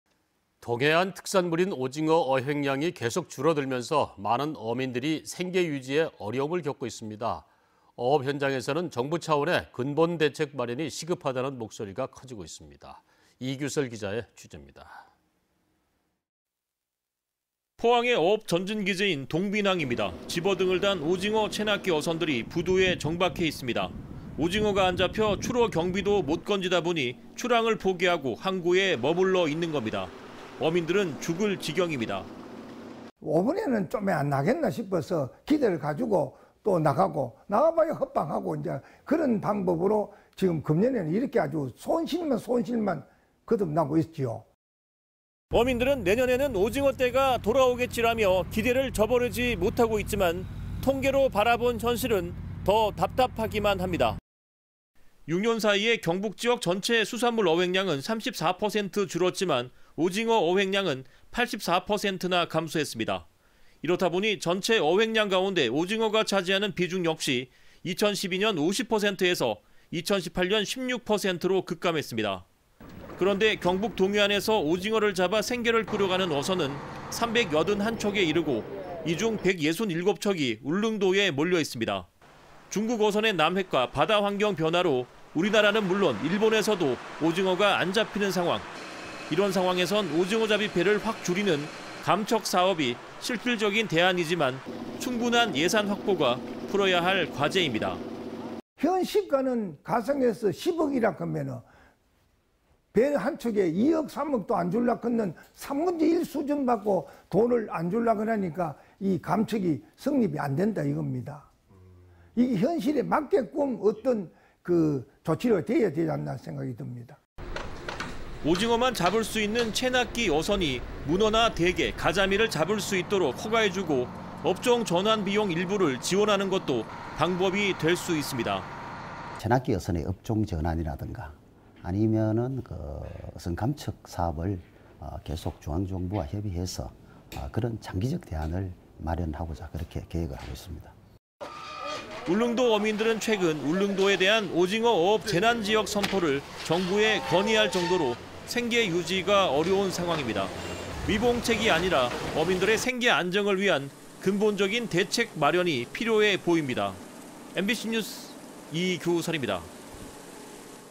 [대구MBC뉴스] 안 잡히는 오징어‥어민 생계 근본대책 필요
내용: 안 잡히는 오징어‥어민 생계 근본대책 필요 - 이규설 기자 (포항M)